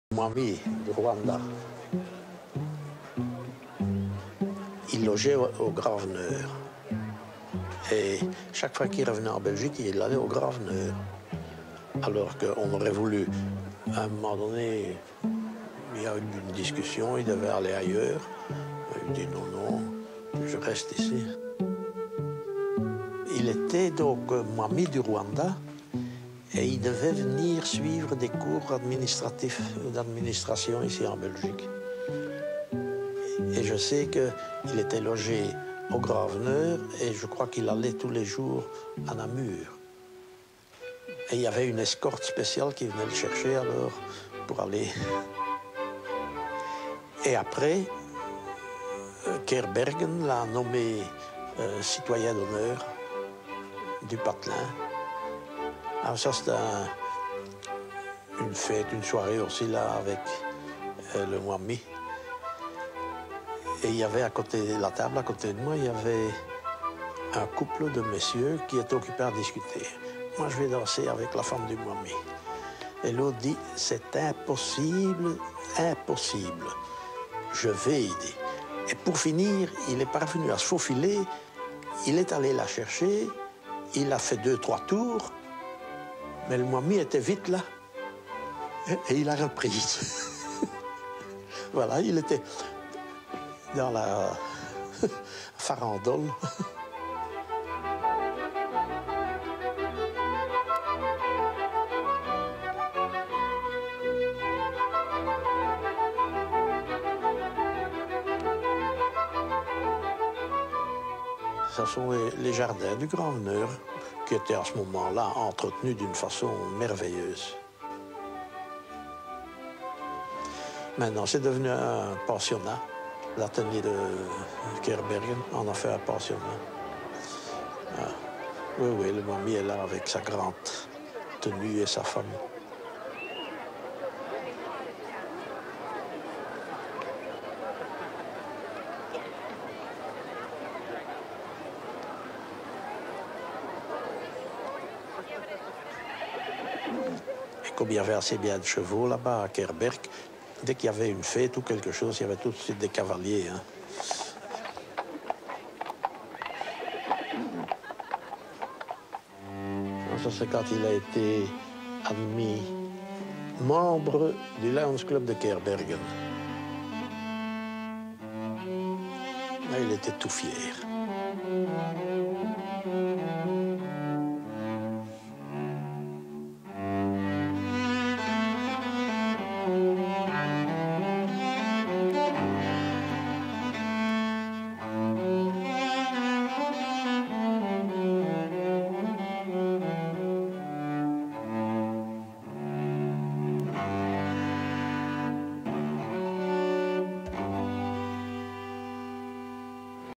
King of Rwanda Mutara III Rudahigwa Grand Veneur
King of Rwanda Mutara III Rudahigwa visits the Grand Veneur in Keerbergen.

King of Rwanda Mutara III Rudahigwa bezoekt de grand Veneur in Keerbergen.